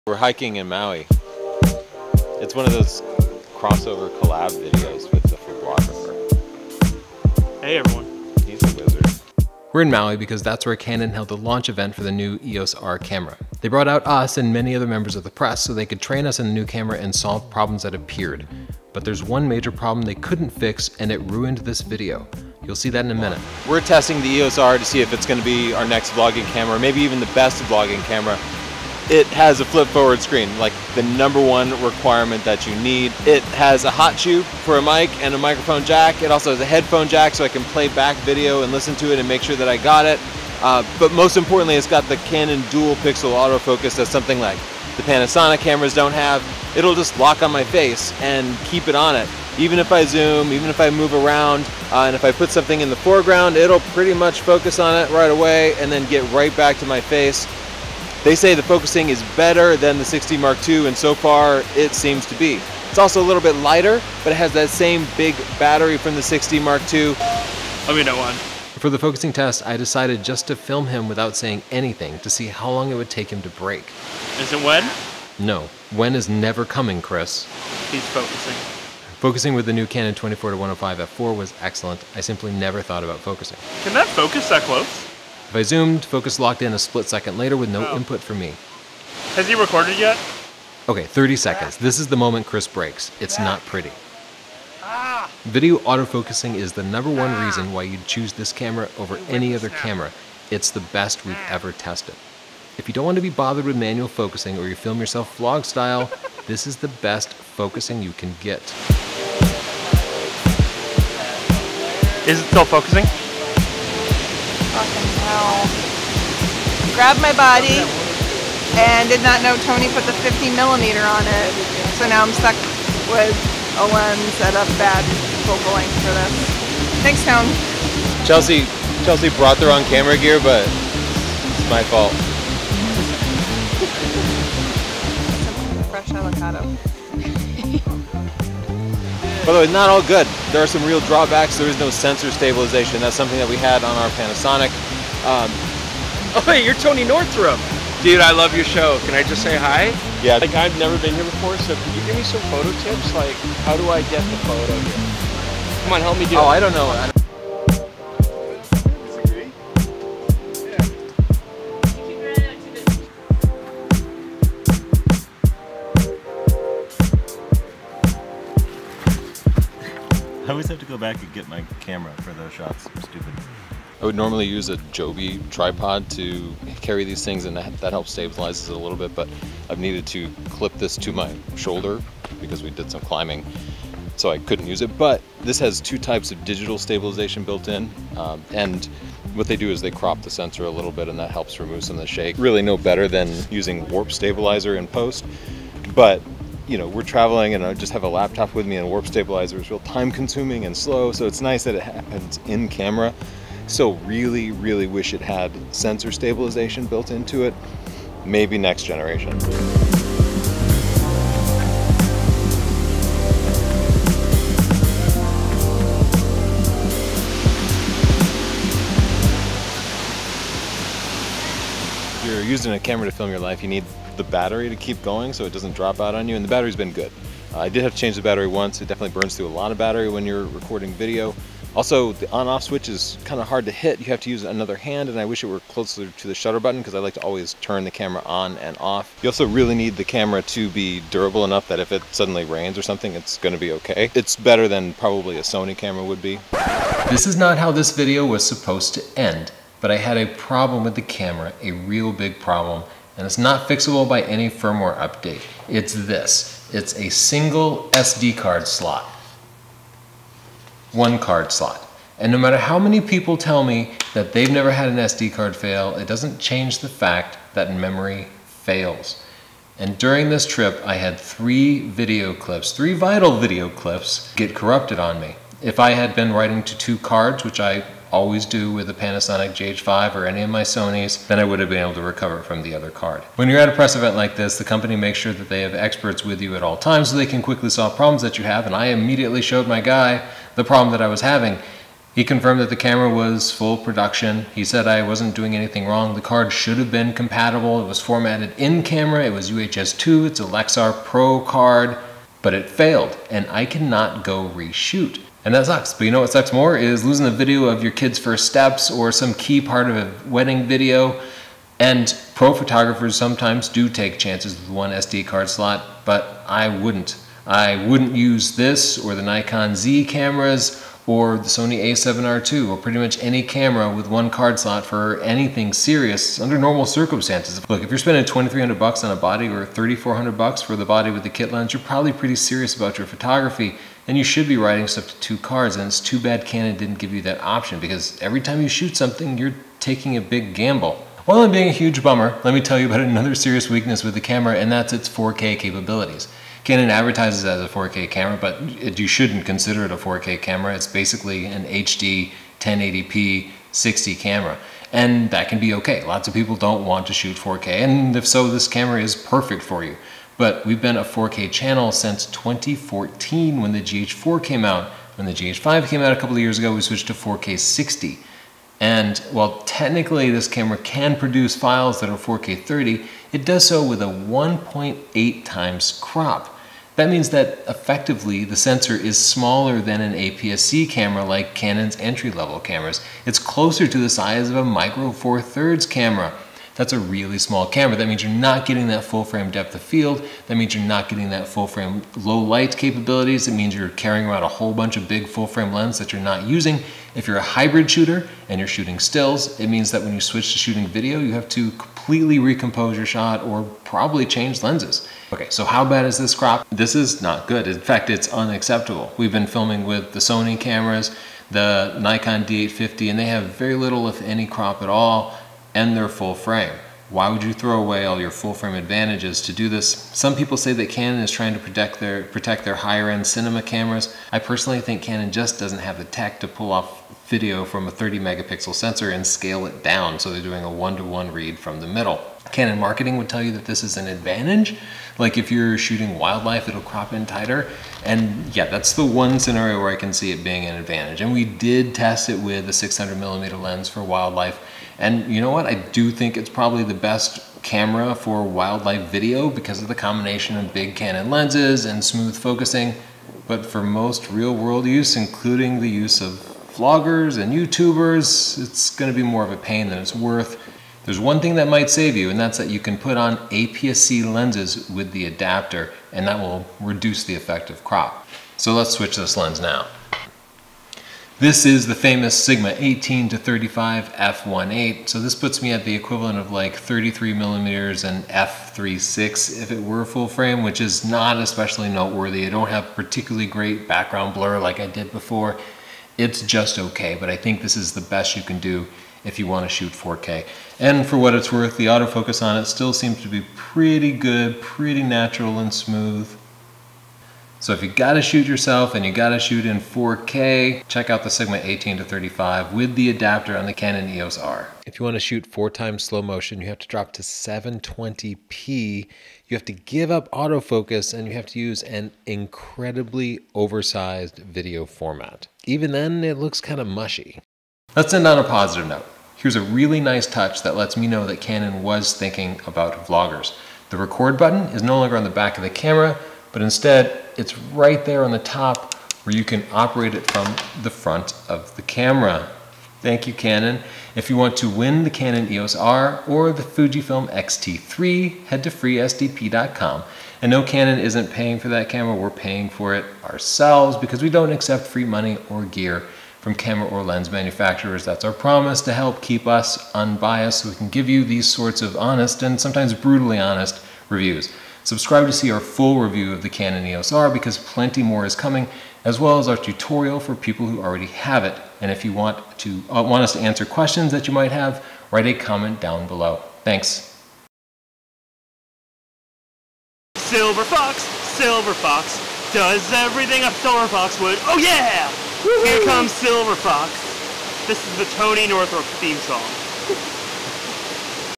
Canon EOS R: Best vlogging camera (is flawed)
is the newest full-frame mirrorless camera. For YouTubers making vlogs, it's probably the best all-around camera with dual-pixel autofocus, a flip screen, weather resistance, and a big battery. However, it has some HUGE drawbacks compared to competing cameras: no sensor stabilization, a ridiculous 1.8X crop on 4k video, and slow-motion only at 720p. We review the Canon EOS R to see if it will be our new vlogging camera.